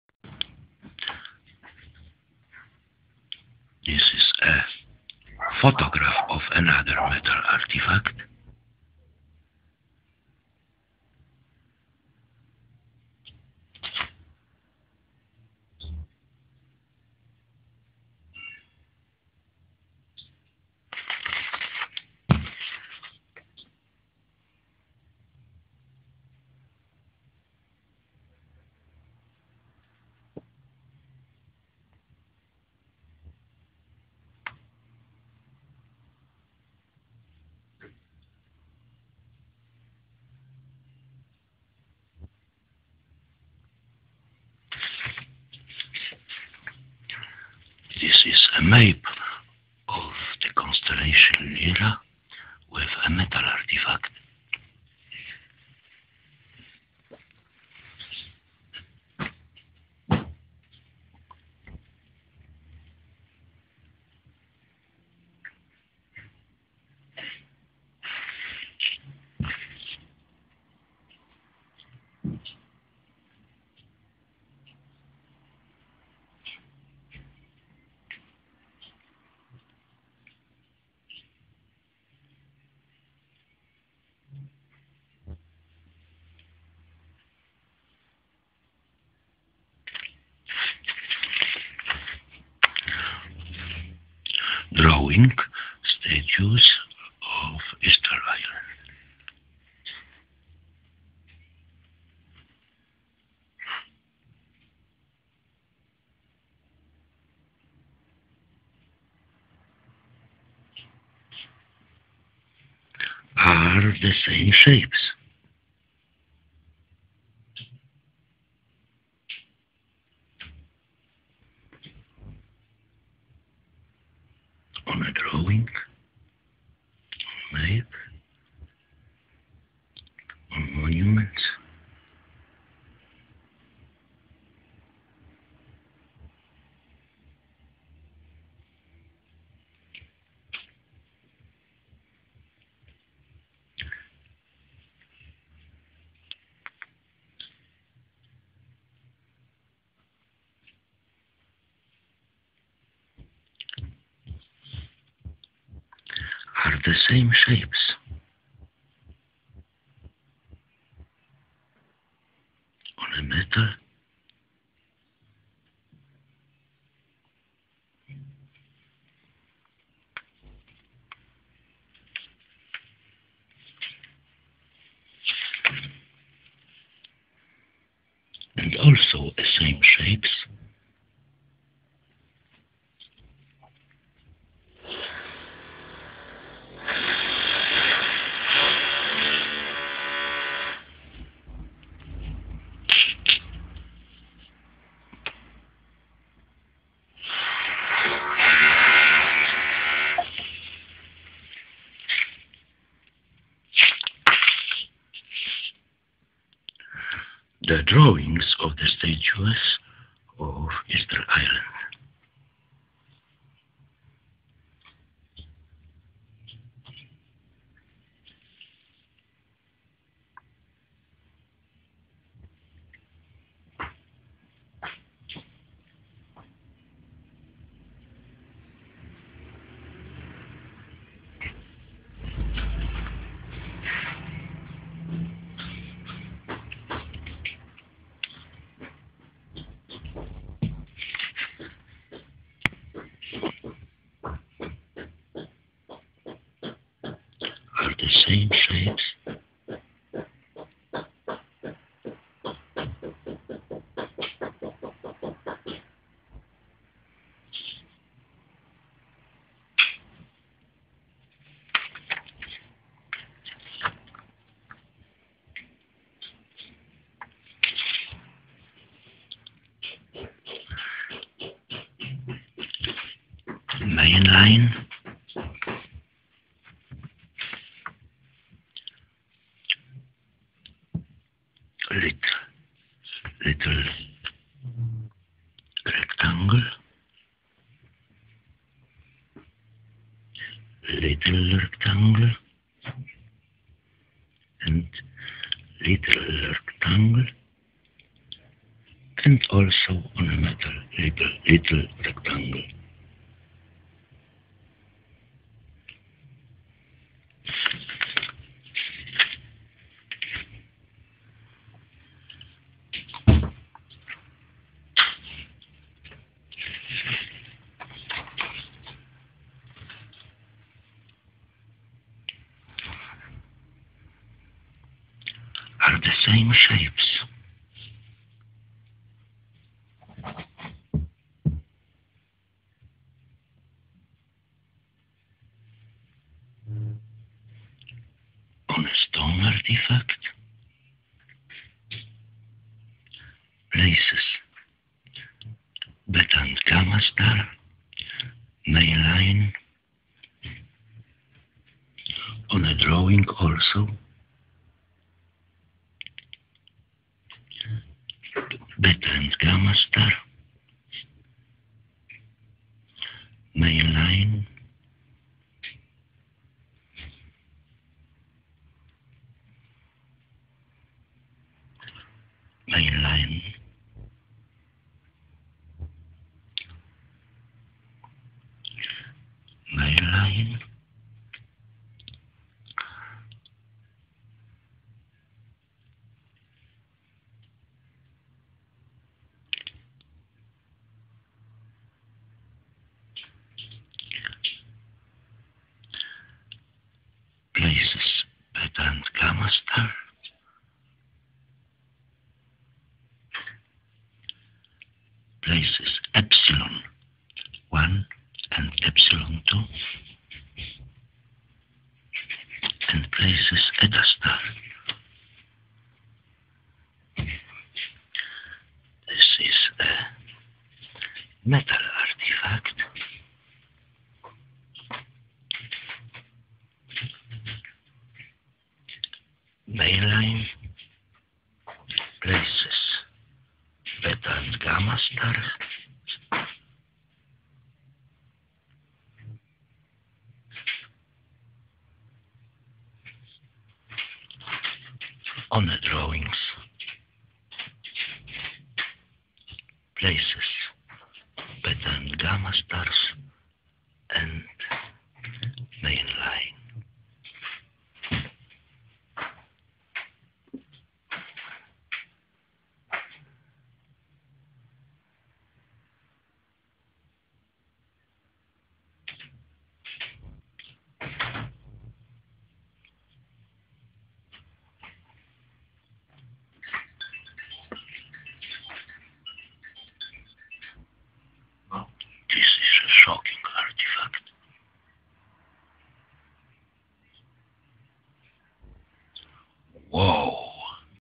Authentic Alien Artifacts From Orion - Explorer B. Marek Biały . Part 13
Issued collection is related to the discovery of all time  / made by the author - B. Marek Biały /
And that our planet was visited by extraterrestrial civilizations and lived  / in Era Amber / - From Constellation ORION.